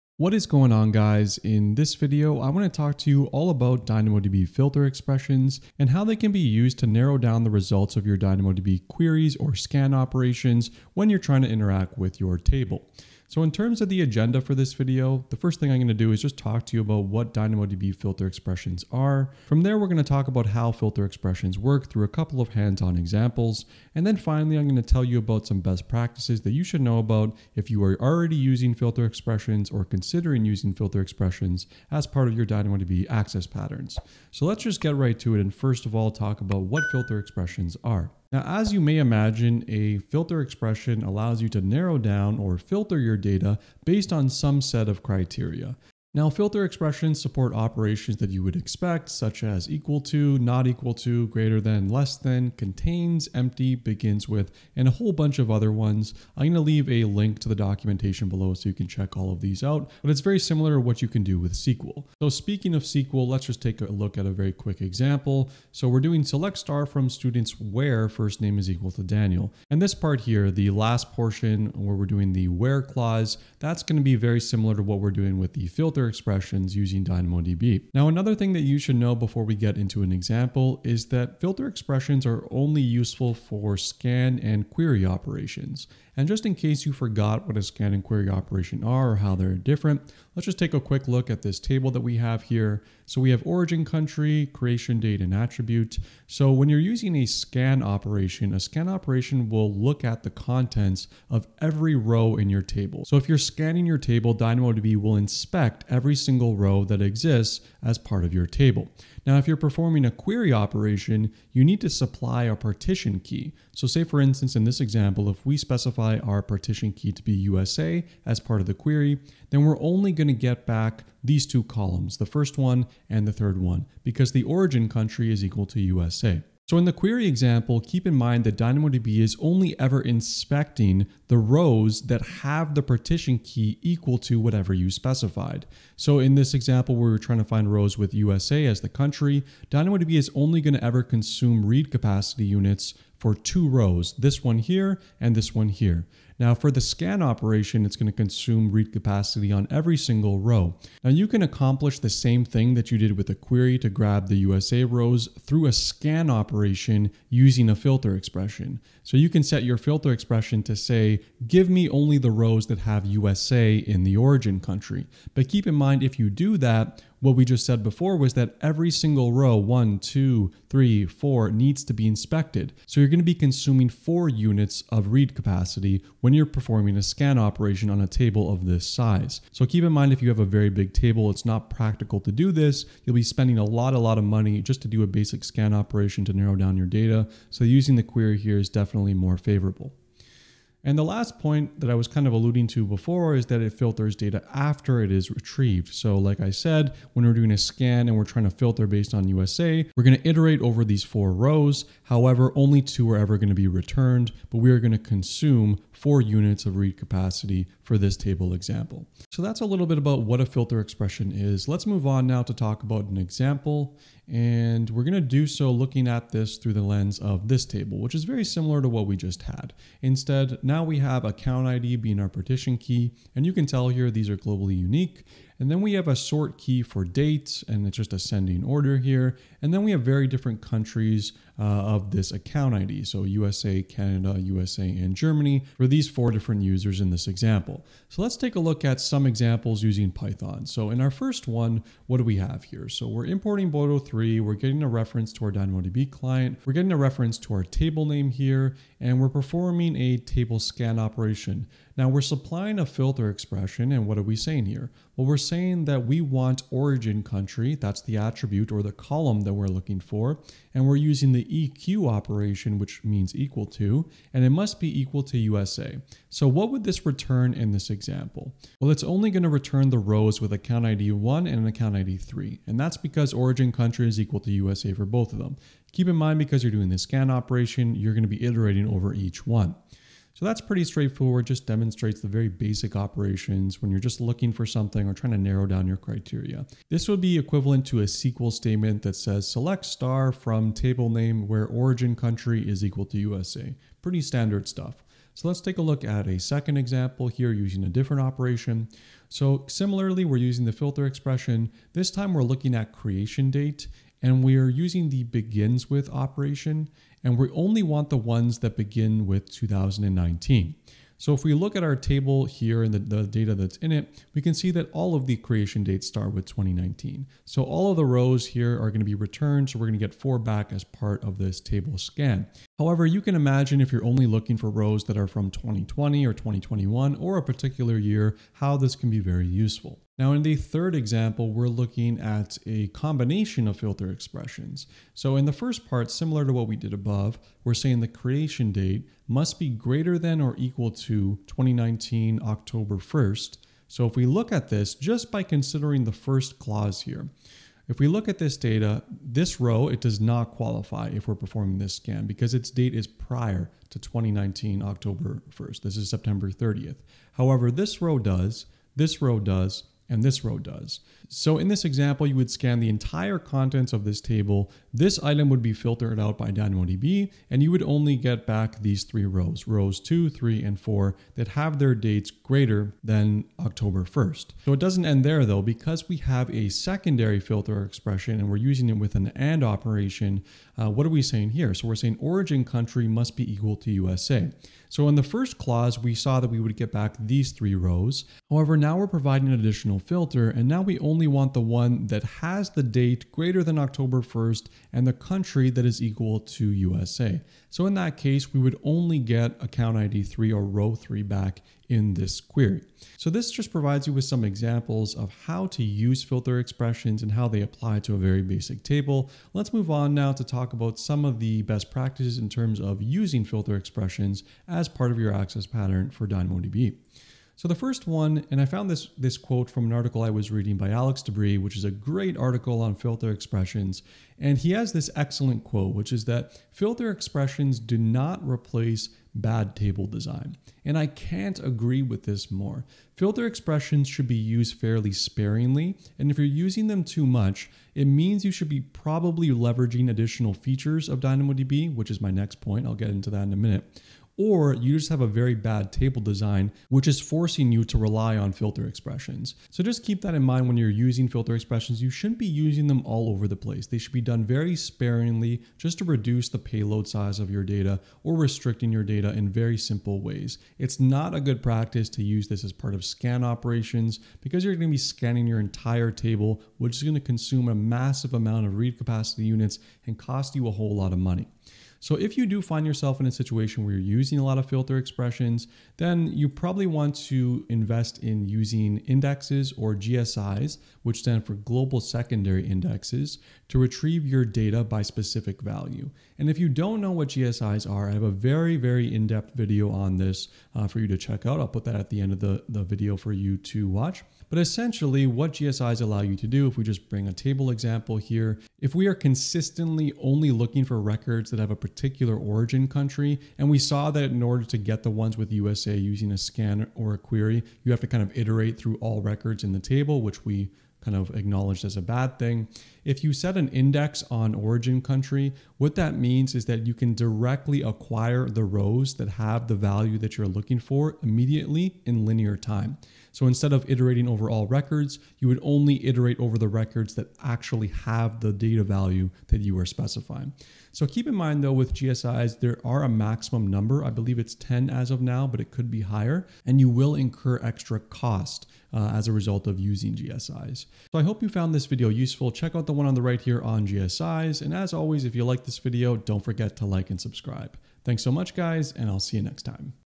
What is a DynamoDB FilterExpression? (for Scan and Query!)
Filter Expressions are DynamoDB's way of restricting data as part of your scan or query operations. They are very similar to WHERE clauses in SQL, but have slightly different behaviour due to the nature of NoSQL. In this video, I tell you about what FilterExpressions are, How they work, some examples, and best practices.

Looking to get hands on experience building on AWS with a REAL project?

Working Effectively with Legacy Code

Filter expression documentation